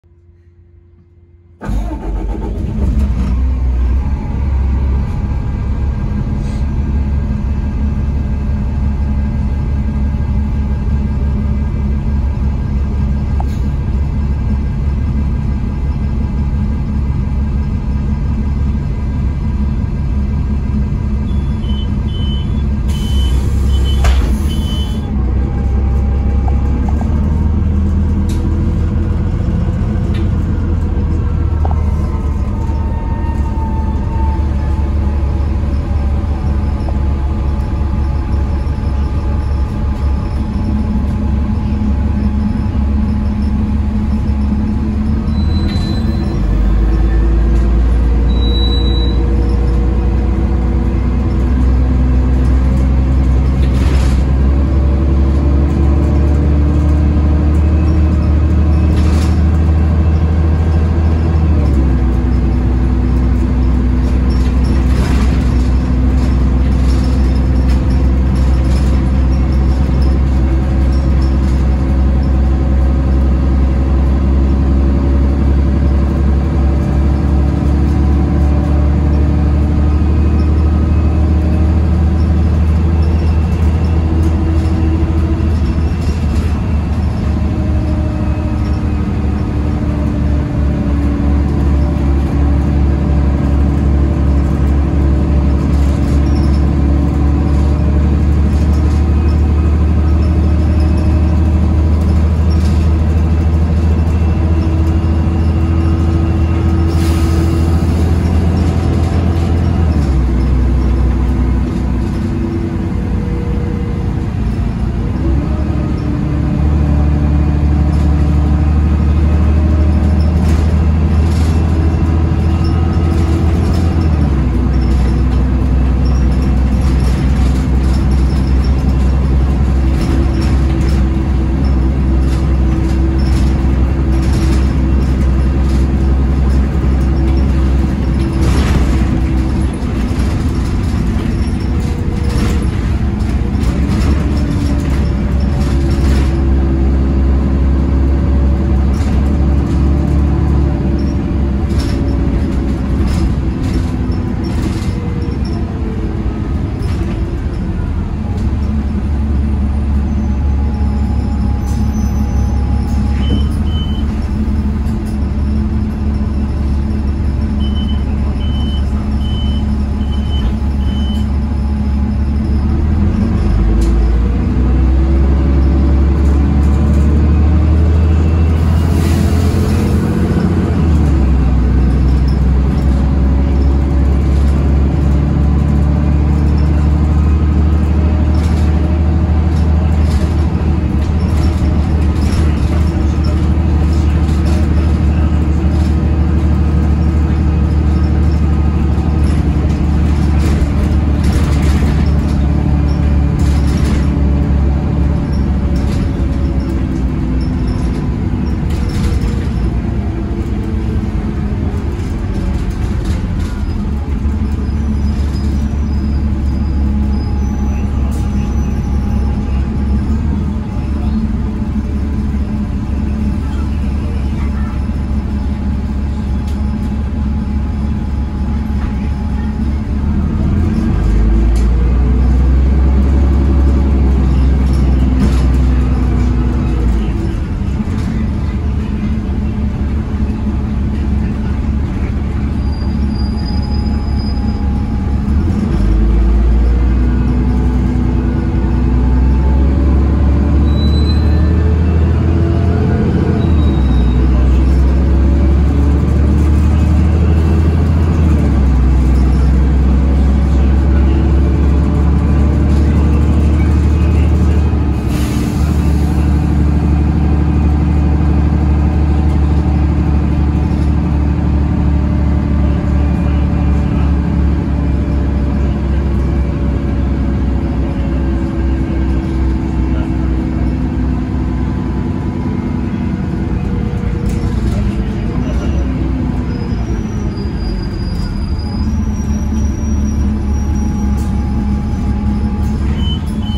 [#72] 519 ➡️ Dw. Centralny (Trasa Zmieniona) 🚍 Solbus Solcity SM18 #2025 - MZA Warszawa
Specyfikacja techniczna
Rok produkcji: 2014
Silnik: Cummins ISB6.7 E5 340B (340 KM)
Skrzynia biegów: VOITH D854.6 (4 biegi)
🔑: Miejskie Zakłady Autobusowe sp. z.o.o. oddz. R-1 „Woronicza”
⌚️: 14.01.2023
Przystanki na trasie:
🚏Powsin-Park Kultury
🚏Gąsek
🚏Andrutowa
🚏Przekorna
🚏Rosochata